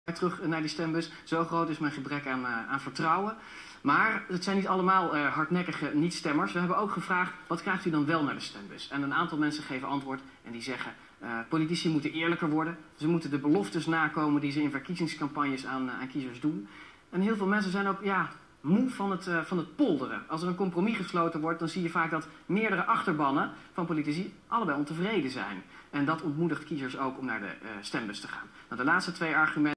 BredaKiest2014 waarom stemmen mensen niet?(4)
1vandaag Opiniepanel (2500) heeft antwoord gegeven waarom men (wel?) niet gaat stemmen.